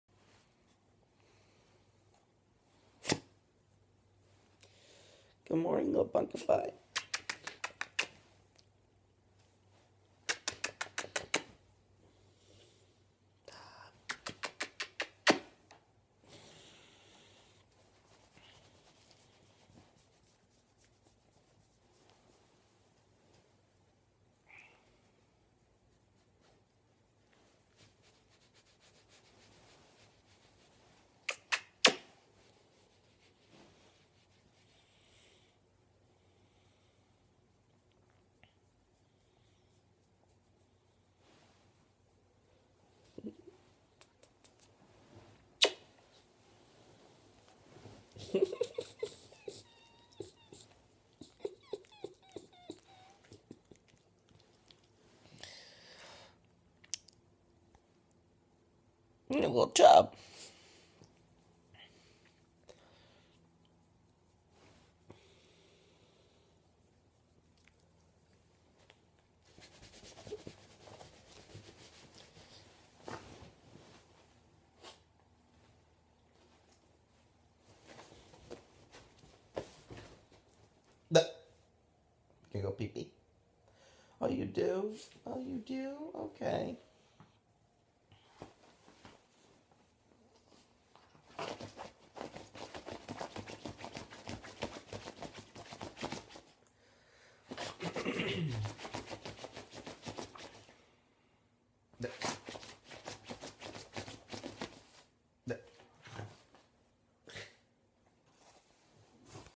Toby the Maltese dog takes forever to wake up
Maltese dog